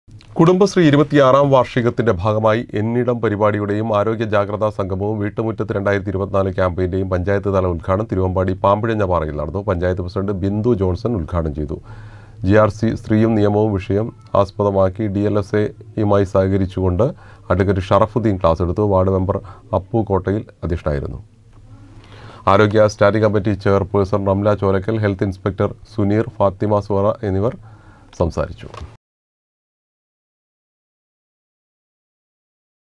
കുടുംബശ്രീ ഇരുപത്തിയാറാം വാർഷികത്തിന്റെ ഭാഗമായി " എന്നിടം " പരിപാടിയുടെയും 'ആരോഗ്യജാഗ്രത സംഗമവും
കുടുംബശ്രീ ഇരുപത്തിയാറാം വാർഷികത്തിന്റെ ഭാഗമായി " എന്നിടം " പരിപാടിയുടെയും 'ആരോഗ്യജാഗ്രത സംഗമവും വീട്ടുമുറ്റത്ത് 2024'ക്യാമ്പയിന്റെയും പഞ്ചായത്ത്‌ തല ഉത്ഘാടനം തിരുവമ്പാടി പാമ്പിഴഞ്ഞ പാറയിൽ നടന്നു